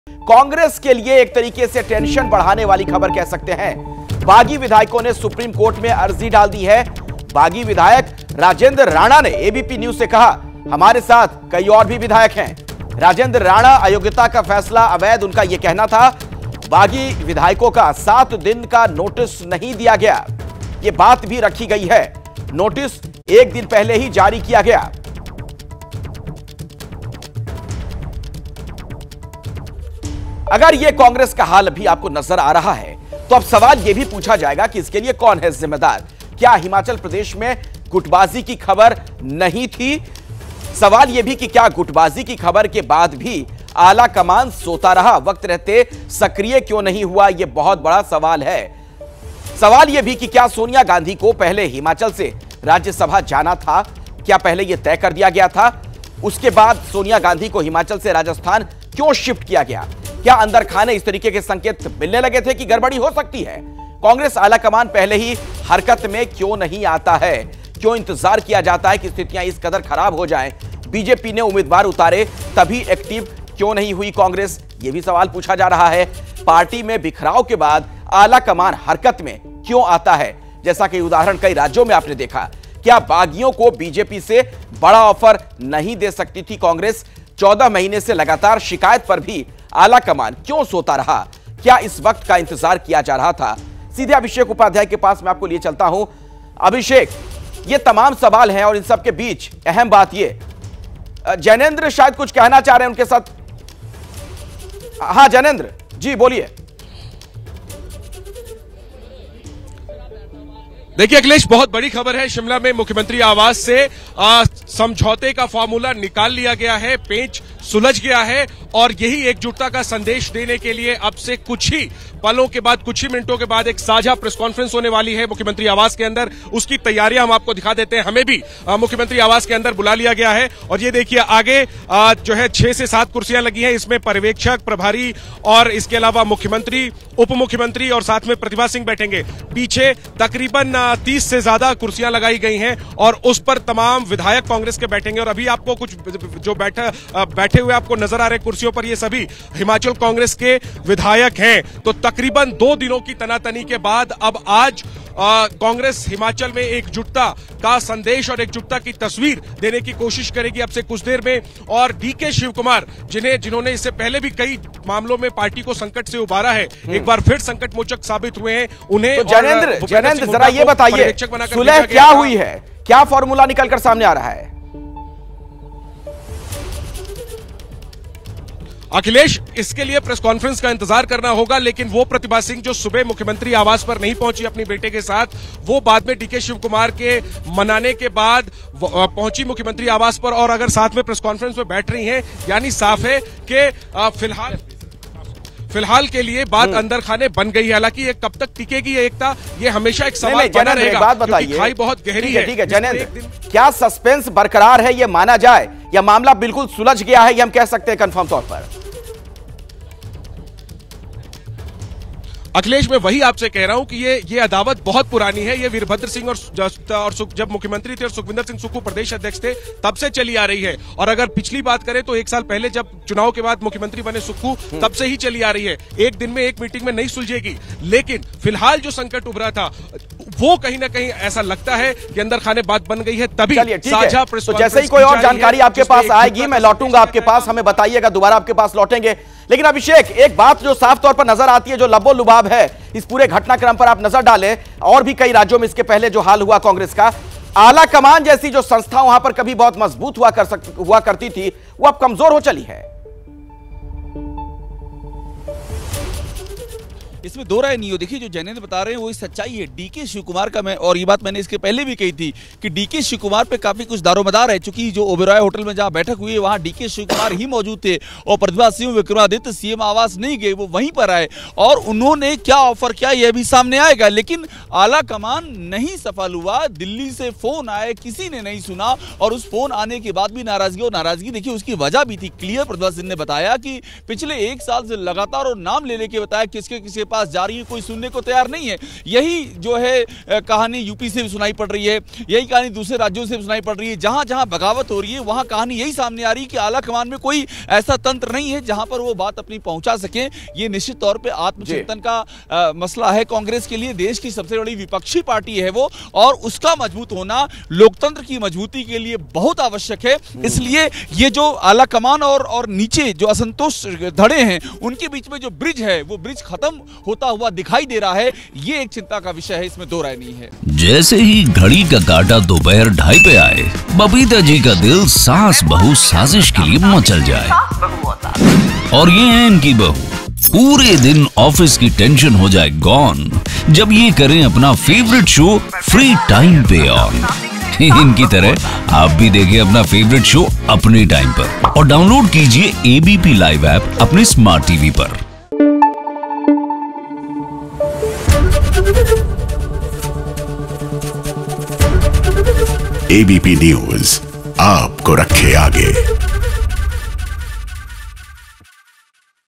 अयोग्य घोषित करने के फैसले के खिलाफ बागी विधायकों ने सुप्रीम कोर्ट का दरवाजा खटखटाया | Breaking
हिमाचल प्रदेश में जारी सियासी संकट के बीच स्पीकर कुलदीप सिंह पठानिया के अयोग्य घोषित करने के फैसले के खिलाफ बाग़ी विधायकों ने सुप्रीम कोर्ट का दरवाजा खटखटाया. राजेंद्र राणा ने एबीपी न्यूज से बात करते हुए दावा किया कि उनके साथ कई विधायक हैं.
विधानसभा के स्पीकर कुलदीप सिंह पठानिया ने गुरुवार को छह कांग्रेस विधायकों को अयोग्य घोषित कर दिया. इन विधायकों ने राज्य की एकमात्र राज्यसभा सीट के लिए हाल ही में हुए चुनाव में ‘क्रॉस वोटिंग’ की थी.
इन विधायकों ने राज्यसभा चुनाव में बीजेपी के उम्मीदवार हर्ष महाजन के पक्ष में ‘क्रॉस वोटिंग’ की थी और बाद में वे विधानसभा में बजट पर मतदान के समय भी अनुपस्थित रहे. अयोग्य घोषित किए गए विधायकों में राजेंद्र राणा, सुधीर शर्मा, इंद्रदत्त लखनपाल, देवेंद्र कुमार भुट्‌टो, रवि ठाकुर और चैतन्य शर्मा शामिल हैं.